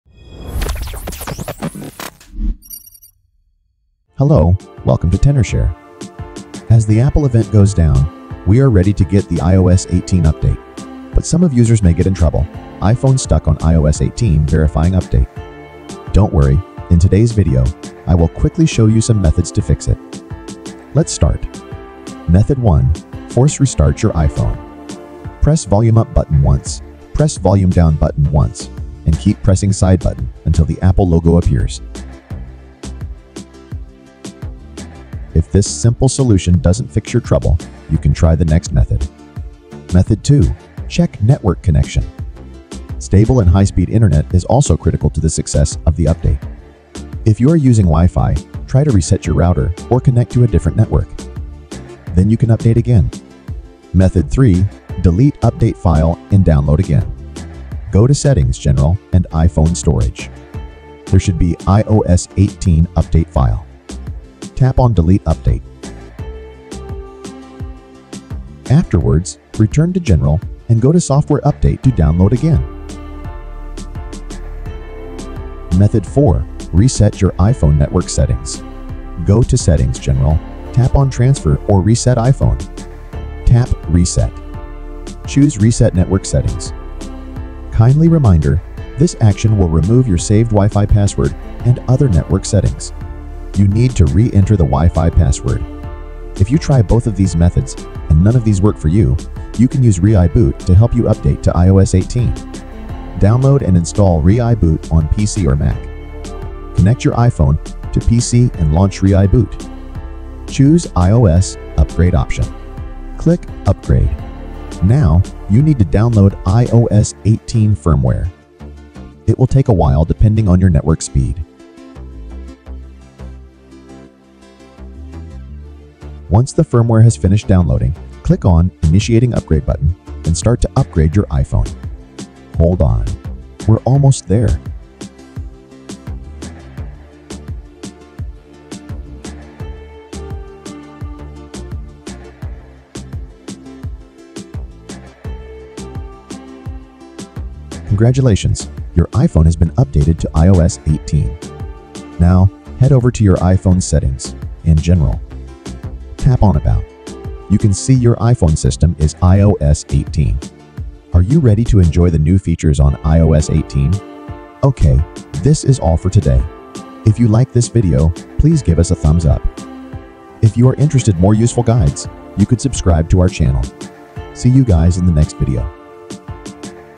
iOS 26/18 Stuck on Verifying Update? Fix it and Update iOS 26/18! - iPhone Stuck on Verifying Update
iPhone stuck on update screen？iPhone Stuck on Verifying Update? Some of users may get in trouble: iPhone stuck on iOS 18.4 verifying update.How to fix this problem? Don't worry! In today's video, I will show you 4 methods to fix it. Let's check out!

*⏱How long does iOS 18 take to install? iOS 18 update taking a long time?*
Based on the actual upgrade experience, the iOS 18 OTA upgrade is expected to take 20-30 minutes. You might want to use iTunes or Finder for a faster iOS 18 upgrade. If you find these software inconvenient, you can consider using ReiBoot to update iOS 18 in 10 minutes.

⏱ *TIMESTAMP*
00:00 Intro
00:22 Method 1: Force Restart Your iPhone
00:39 Method 2: Check Network Connection
00:53 Method 3: Delete Update File and Download Again
01:15 Method 4: Reset Your iPhone Network Settings
01:39 Method 5: Update to iOS 18 with ReiBoot in 10 minutes

💡 *How To Fix iPhone Stuck on iOS 18 Verifying Update*
Method 1: Force Restart Your iPhone
Press the volume up button once.
Press the volume down button once.
Hold the side button until the Apple logo appears.
If this simple restart doesn’t resolve the issue, move on to the next method

Method 2: Check Network Connection
A stable and high-speed internet connection is essential for a successful update.
If you’re on Wi-Fi, reset your router, or try connecting to a different network.
Then, you can attempt to update your iPhone again.

Method 3: Delete Update File and Download Again
Go to Settings - General - iPhone Storage.
Look for the iOS 18 Update File and tap on Delete Update.
Afterwards, return to General - Software Update, and download the update again.

Method 4: Reset Your iPhone’s Network Settings
Go to Settings - General - Transfer or Reset iPhone.
Tap Reset and choose Reset Network Settings.
Note: This will remove all your saved Wi-Fi passwords, so you’ll need to reconnect manually.

📑 *Tenorshare Official Article Links*
Fix iPhone Stuck on Verifying Update iOS 17/18 with Video Guide